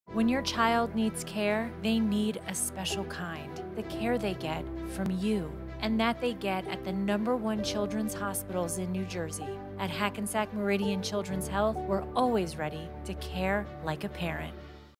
0:15 Hackensack Meridian Children's Health - Care Like a Parent
You deserve a healthcare provider as dedicated to your child's well-being as you are. At the #1 Children's Hospitals in NJ, ranked by U.S. News & World Report, our patients and families receive a special kind of care--unwavering, unstoppable, "not on my watch" care. To learn more about our nationally-ranked pediatric specialties, physicians and surgeons,